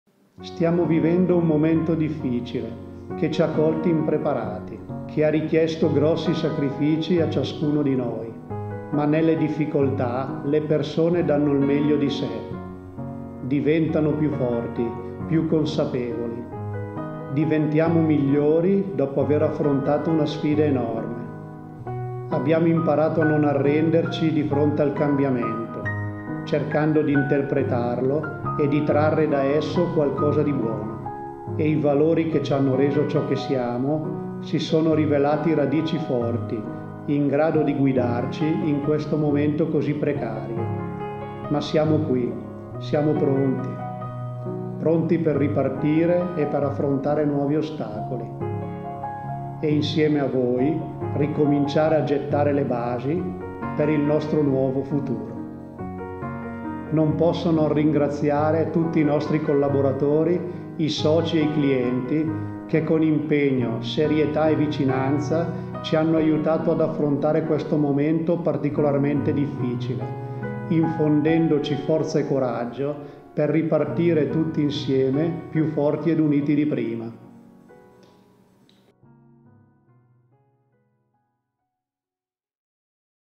Bcc Treviglio | Ripartiamo - Insieme Per Il Futuro
Ci siamo, siamo pronti. Pronti per riprendere in mano le nostre vite, fare la differenza, dimostrare ancora una volta la nostra forza e gettare le basi del nostro nuovo futuro.
E, soprattutto, siamo pronti per farlo insieme

Ecco il messaggio della BCC di Treviglio accompagnato dalla voce di Giovanni Grazioli